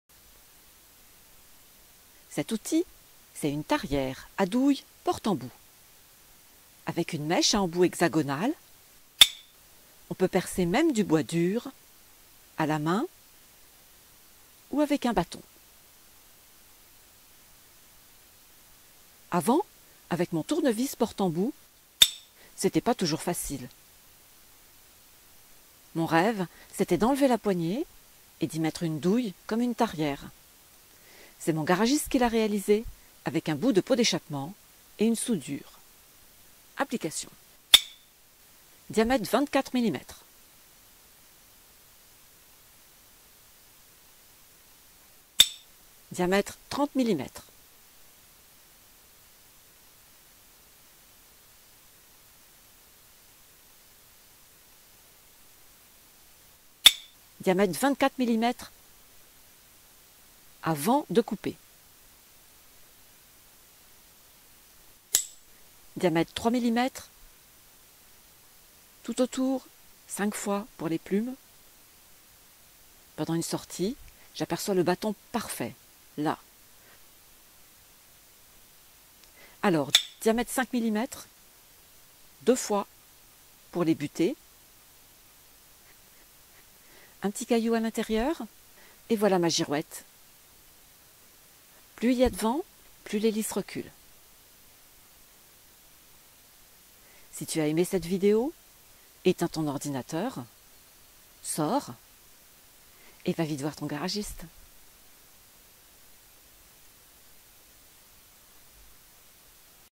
La tarière à douille porte-embout et sa girouette.
La tarière à douille porte-embout : à quoi ça sert, fabrication, et utilisation pour faire une girouette.